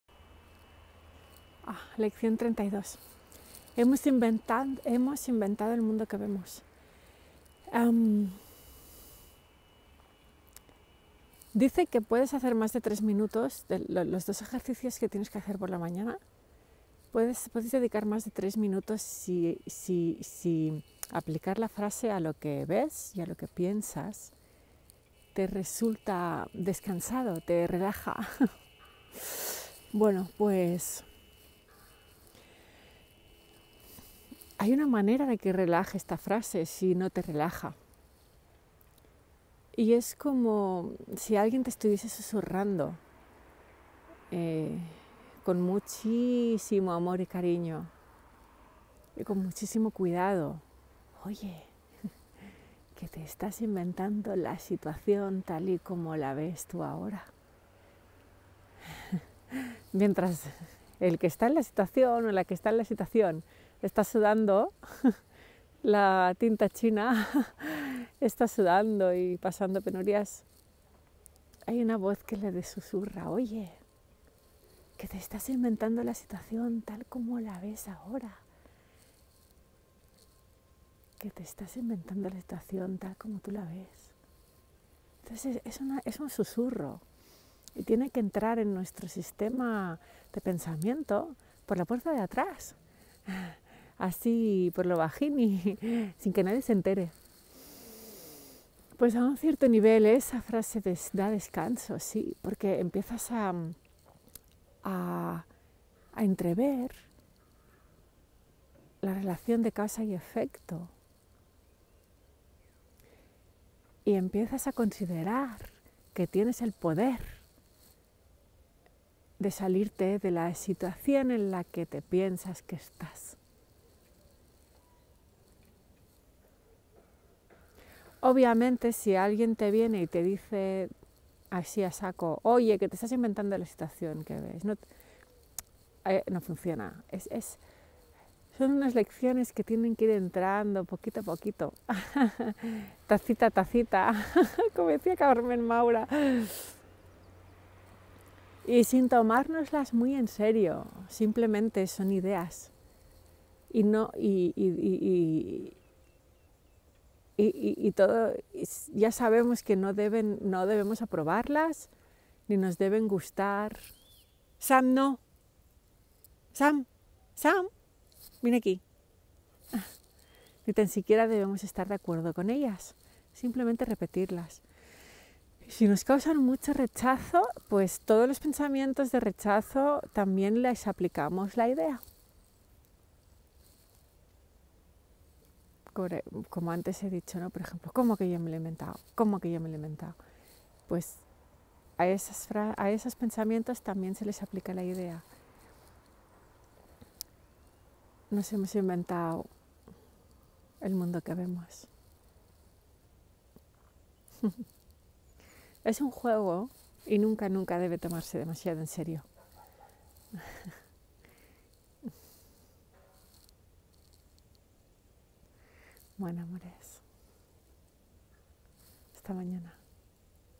Lección 32.Un Curso de Milagros UCDM.A Course in Miracles ACIM.Marta Andreu Guiu.2ªVUELTA. Parte2/2.
Empieza a estudiar UN CURSO DE MILAGROS VIERNES 1 y VIERNES 8 y SABADO 9 de FEBRERO del 2019 a las 18:30 en Llibreria Te Quiero c/torrijos 9 Barcelona.

Twiter: martaguiu33
Aplicación de las lecciones en la vida cotidiana.
SEGUNDA VUELTA

LECCIÓN 32
He inventado el mundo que veo.
I have invented the world I see.